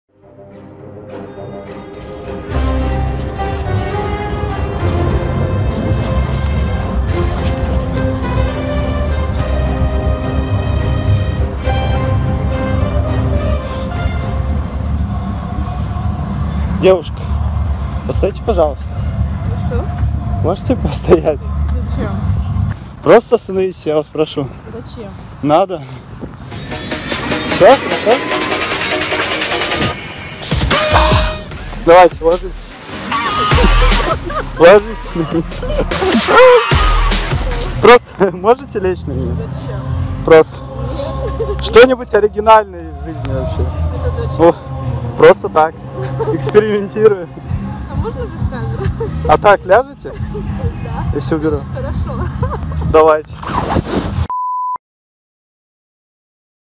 КАК ЗАСТАВИТЬ НЕЗНАКОМУЮ ДЕВУШКУ ЛЕЧЬ НА ТЕБЯ? РАБОТАЕТ 100% | ПРАНК
В этом видео, можно увидеть как заставить незнакомку лечь на тебя, реально рабочий способ, проверено.

пранк пикап